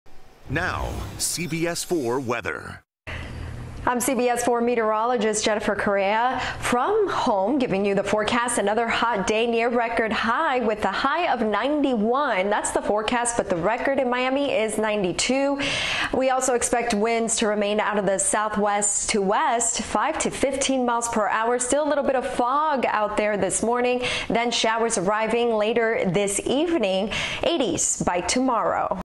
Your CBS4 Forecast For Friday 4/10
CBS4 meteorologist Jennifer Correa says today will be partly cloudy and a stray shower or two is possible. High 91F.